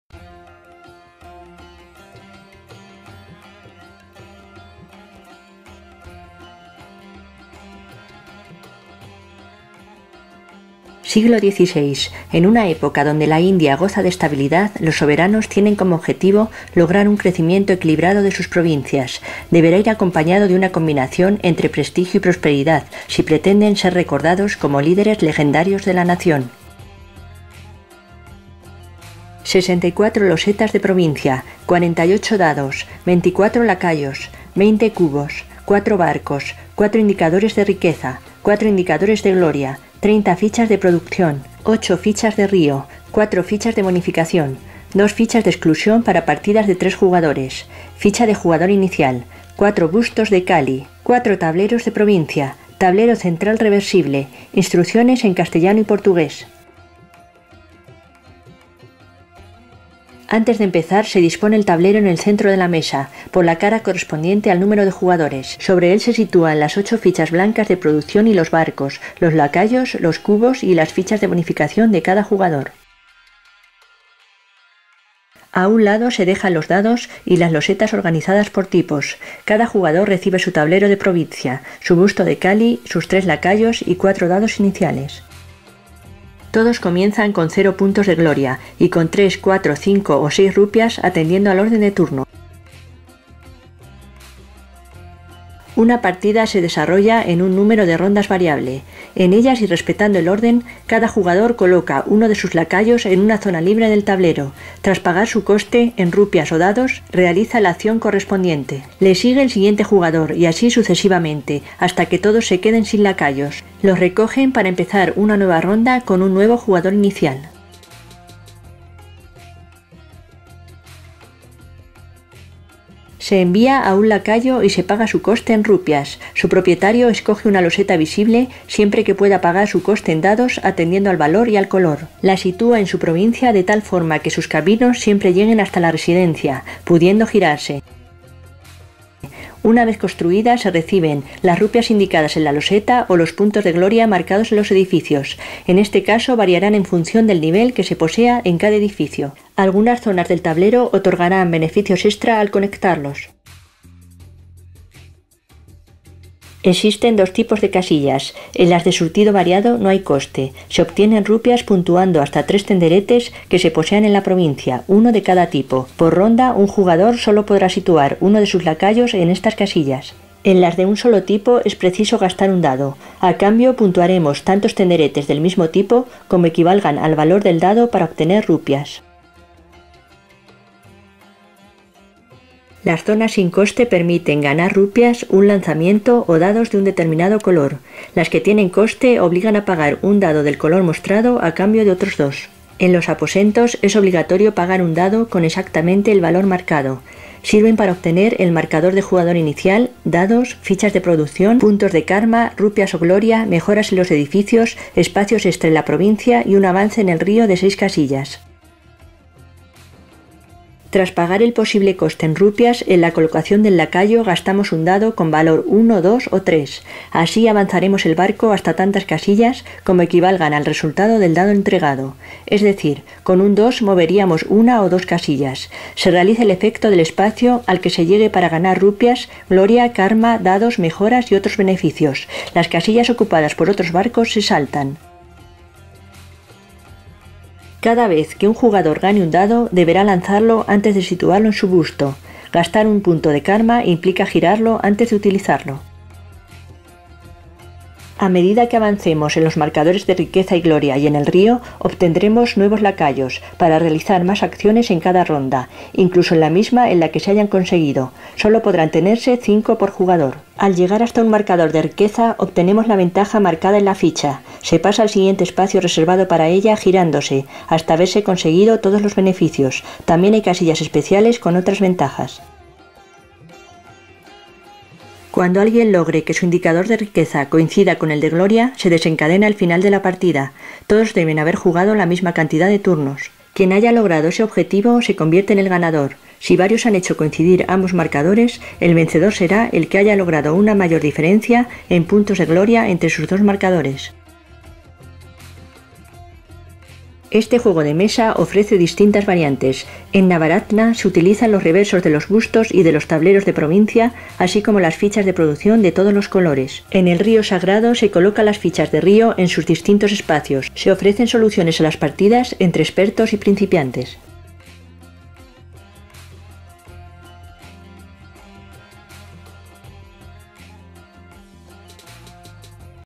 Ganges Vídeo Tutorial
Ganges (Rajas of the Ganges) es un juego de mesa de 2 a 4 jugadores, con partidas de 45 a 75 minutos de duración. Recomendado a partir de los 12 años, es obra de Inka Brand y Markus Brand. Ambos autores son responsables de la serie de juegos Exit, La Villa y La Torre Encantada, entre otros.

Con ilustraciones de Dennis Lohausen, es una publicación original de Huch! Su edición en castellano y portugués corre a cargo de Devir, convirtiéndose en uno de sus juegos más esperados. Ya está disponible.